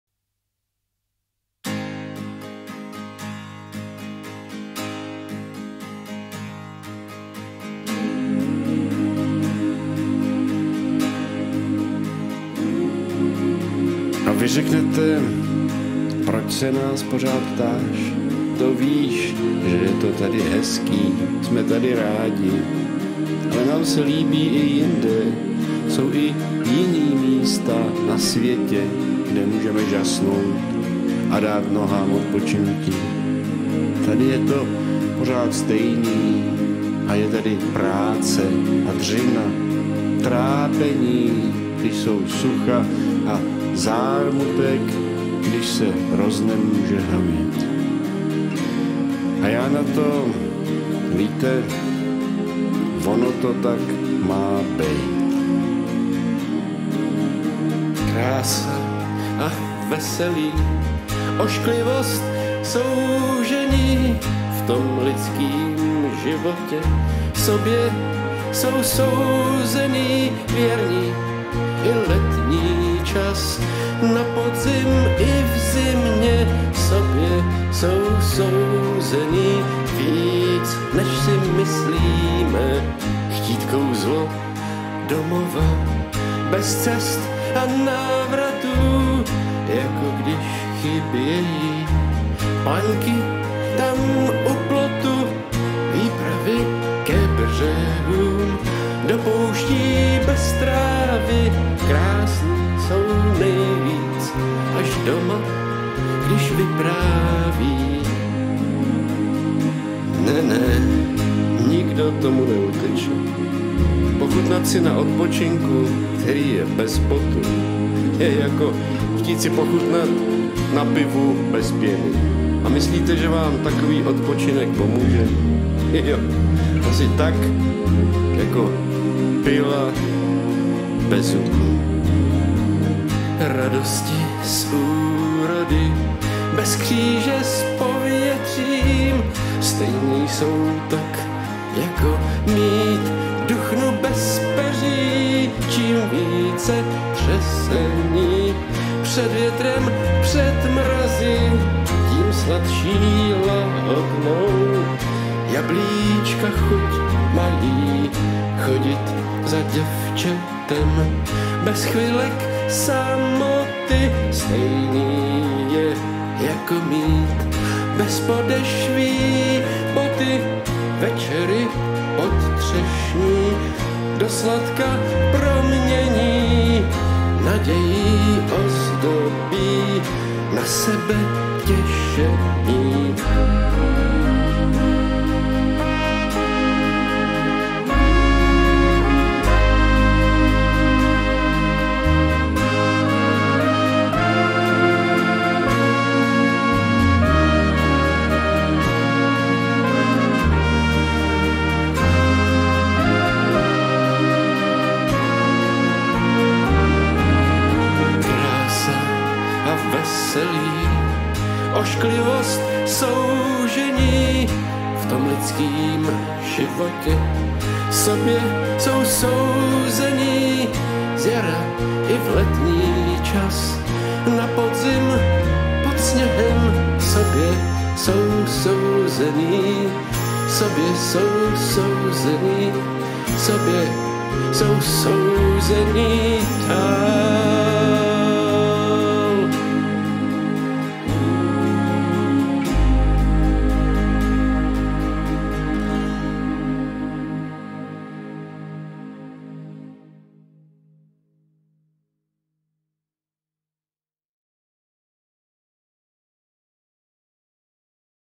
Kouzlo /zkoušky /smysl /zkušenost
/Jakub1,2/ Za největší radost mějte, bratří moji, kdyžkoli v rozličná pokušení upadáte, 3Vědouce, že zkušení víry vaší působí trpělivost. 4Trpělivost pak ať má dokonalý skutek, abyste byli dokonalí a celí, v ničemž nemajíce nedostatku.

Odvěká touha člověka trvale odpočívat je chytání větru.
Inu, stále je o čem dumat.